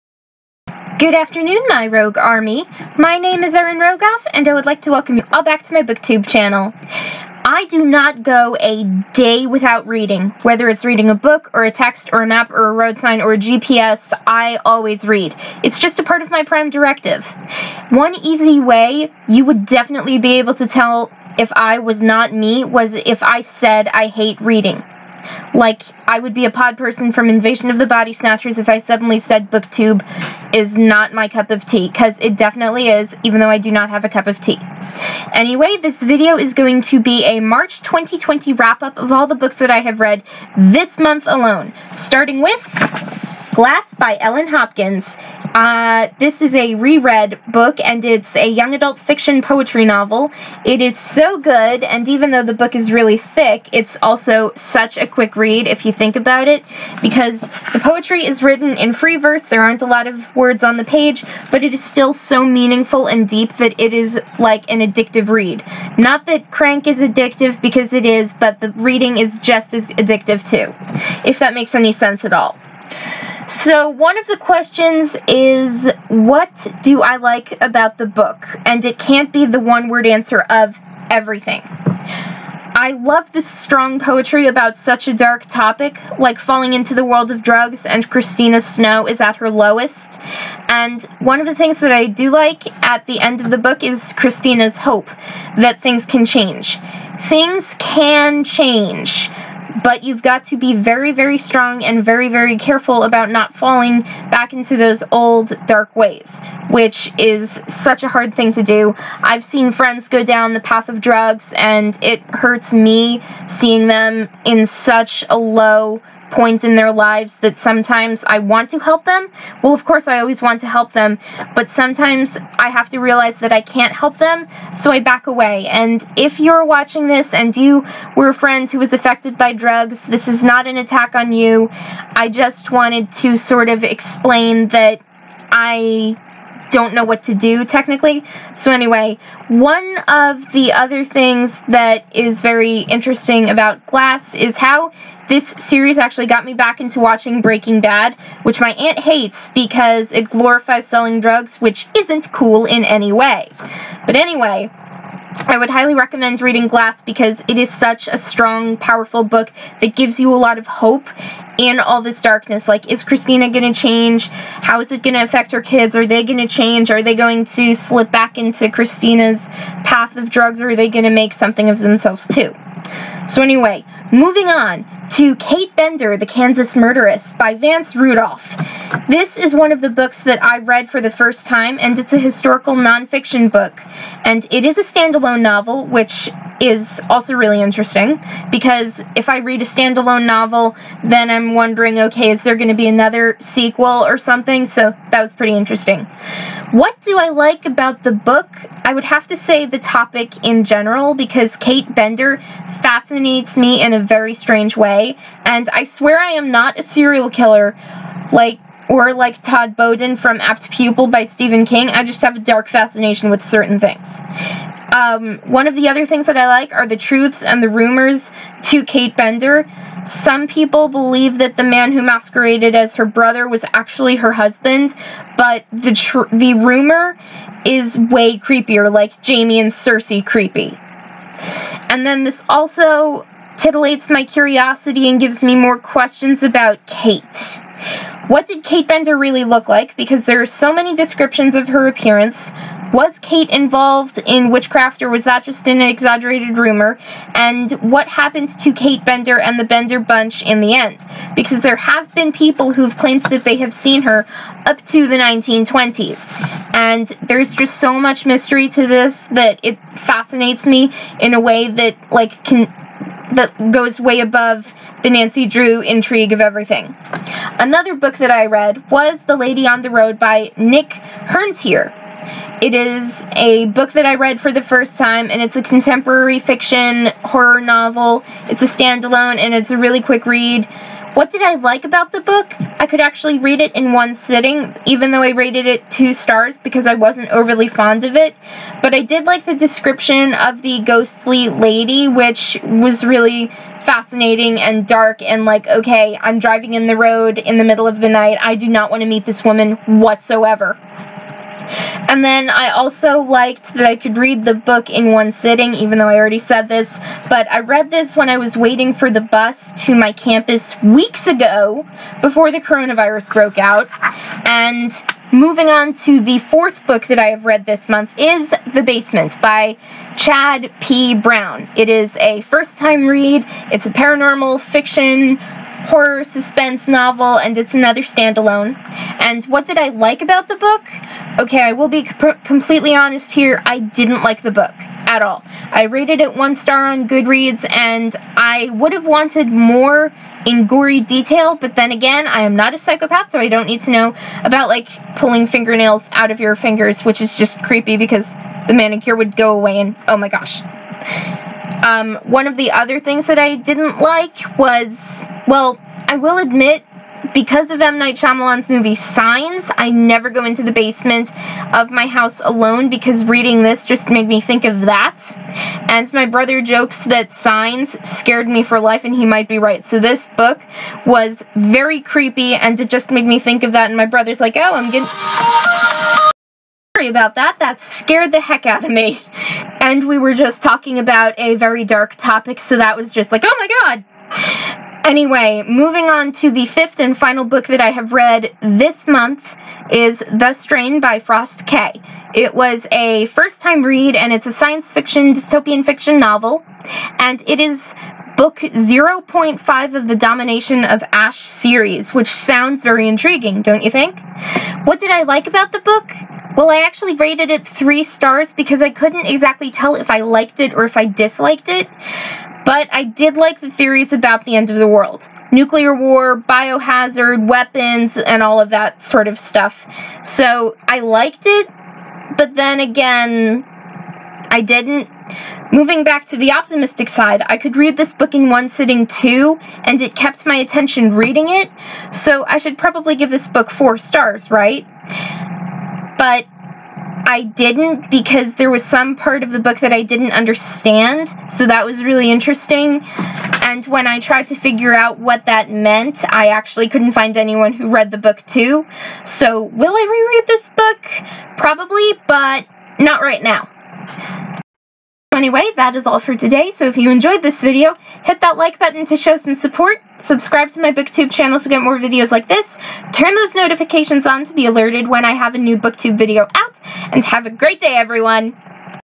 March 2020 Book Wrap-Up (Read 5 Books This Month Alone!)
With all the craziness going on (family problems, a worldwide epidemic, social isolation, etc.), I thought I'd make a March 2020 book wrap-up. And no, even I can't believe I read a total of 5 books this month! Bragging rights granted - - how many books did YOU read this month?